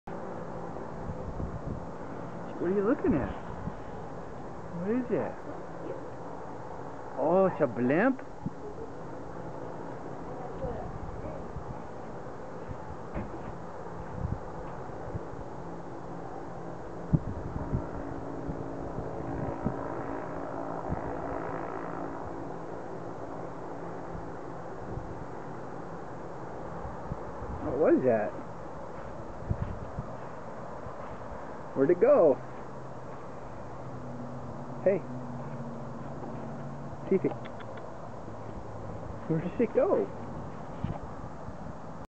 Fifi and the Dirigible
Fifi checks out the Met Life Snoopy blimp